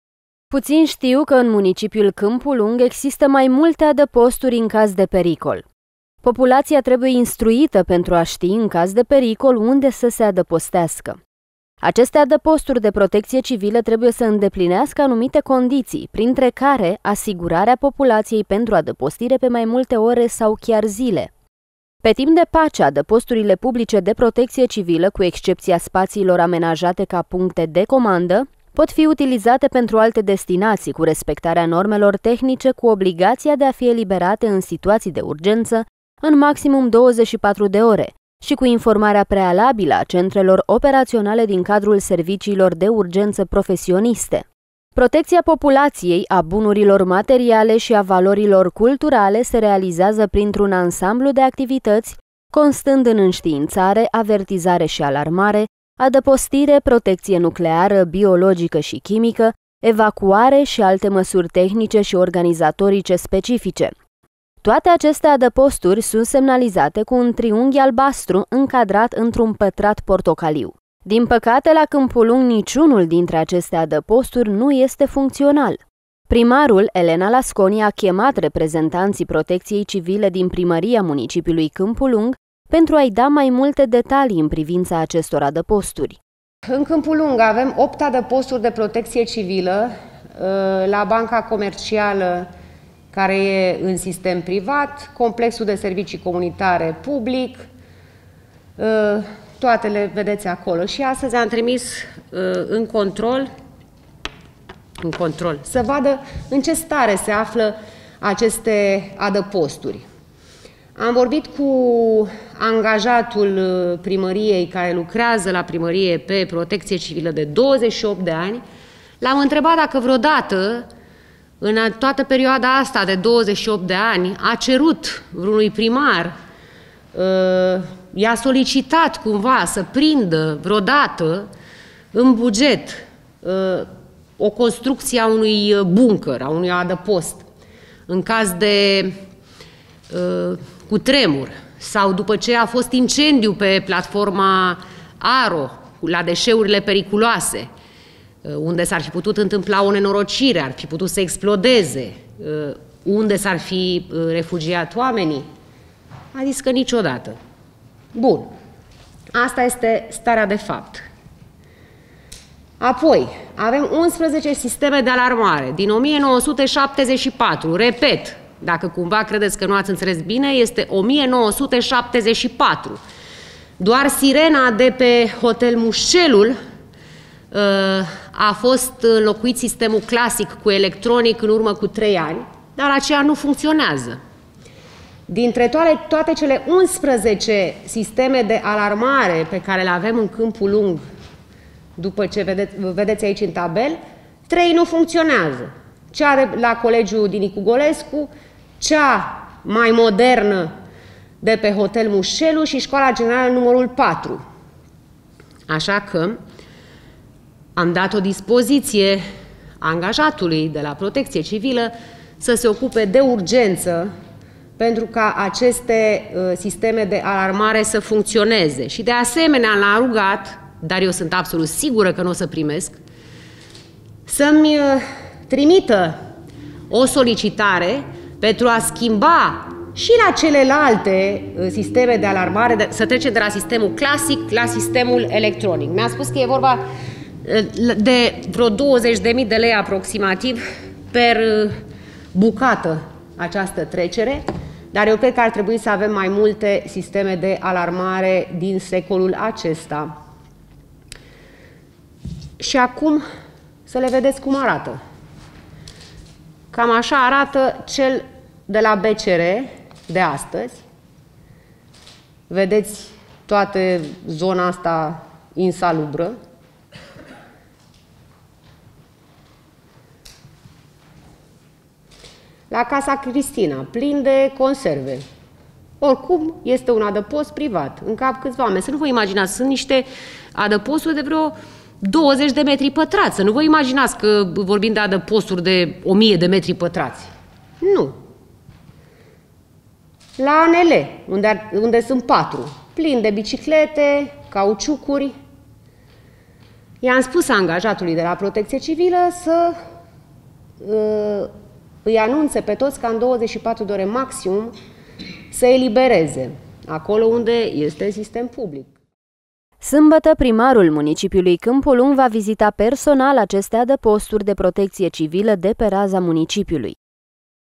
Opt adăposturi în caz de pericol, la Câmpulung ( 25 02 2022 )
Scurtă descriere:

La Câmpulung există opt adăposturi de protecţie civilă, în caz de pericol. Acestea ar fi trebuit să fie dotate cu tot ce este necesar pentru a supravieţui de la câteva ore până la mai multe zile. La Câmpulung, din păcate, în acest moment, niciunul dintre aceste adăposturi nu este funcţional.